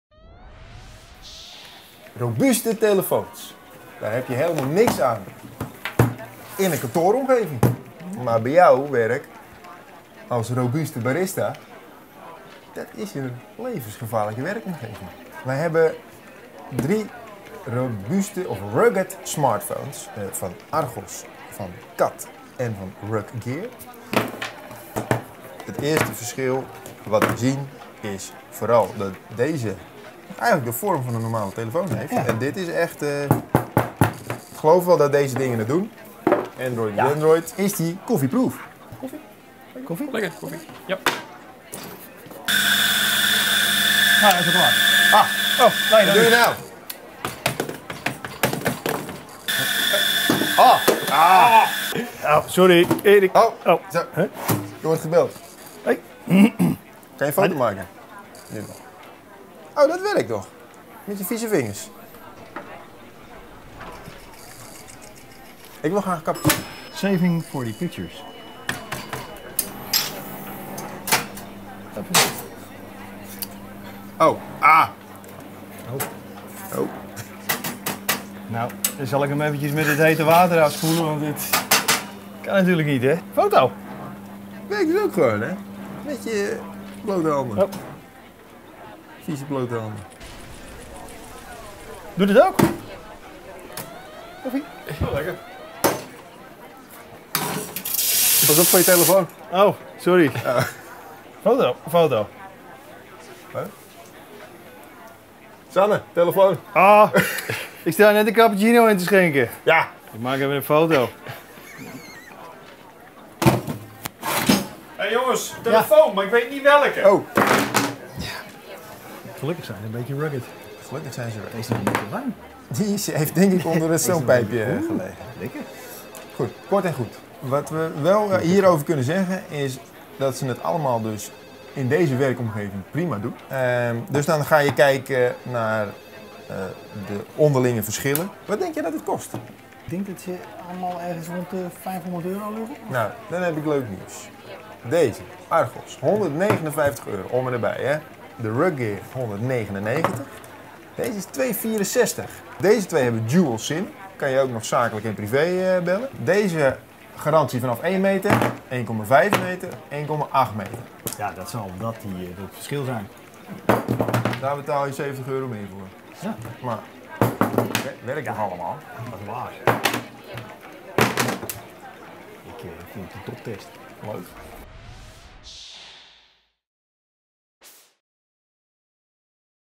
Rugged smartphones in de test
Vroeger ging je niet van huis zonder je sleutels en portemonnee. Tegenwoordig neem je vooral je smartphone mee. En die neem je ook overal mee naartoe, ook naar plekken waar dat misschien niet heel handig is. Ze lopen nogal eens tegen schade op; die toestellen kunnen weinig hebben, ongeacht het prijskaartje. Sterker nog, als je je dure smartphone laat vallen, is de reparatie vaak ook duur.

Gelukkig zijn er steeds meer robuustere exemplaren op de markt. Waar ze voorheen werden gekenmerkt door een grote rubberen rand om het scherm, met veel uitstulpingen om de klappen op te vangen, worden ze tegenwoordig qua design een stuk knapper. De specificaties moet je niet direct vergelijken met de high-end toestellen; ze zijn voor een andere doelgroep en gebruik bedoeld. Wil niet zeggen dat je een minderwaardige smartphone hebt dan, alleen zijn de toeters en bellen anders. Jouw iPhone kan bijvoorbeeld niet uren onder water, of gebruikt worden in een beschermde lab-omgeving vanwege de straling en een noodknoop aan de zijkant om je collega's snel in te lichten ontbreekt ook. Zaken die op rugged smartphones wel te vinden zijn.

We vergelijken drie exemplaren: de RugGear RG650, de CAT S41 en de Archos 50 Saphir. Aangezien ons kantoor weinig fysieke uitdagingen kent, gaven we ze aan Sanne. Hij is barista en daar komen natuurlijk allerlei gevaren bij om de hoek kijken.

Alle toestellen draaien op Android - we hebben bewust niet gekeken naar die specificaties, want daar zijn andere partijen veel beter is. Wij kijken zoals altijd naar de gebruiksvriendelijkheid en de eerste indruk. En die is bij alle toestellen goed. Ze doorstaan de koffietest. Als we kijken naar looks en design lopen de meningen uiteen. Het RugGear-toestel heeft een ontwerp dat kan concurreren met normale smartphones, terwijl het veel robuuster aanvoelt en in de praktijk ook is natuurlijk. De Archos en CAT hebben duidelijk een stootrand. Dat ziet er stoer uit, maar wijkt wel af van het gebruikelijke.

Deze toestellen zullen niet dagelijks onder de koffie worden gedompeld, maar het is fijn om te weten dat je telefoon het kan hebben, mocht dat wel gebeuren. Er zijn genoeg mensen die op een werkplek aan de slag zijn waar stof, water en allerhande andere risico's op de loer liggen. Zonde als je telefoon daardoor schade oploopt. Met deze smartphones hoef je daar niet bang voor te zijn.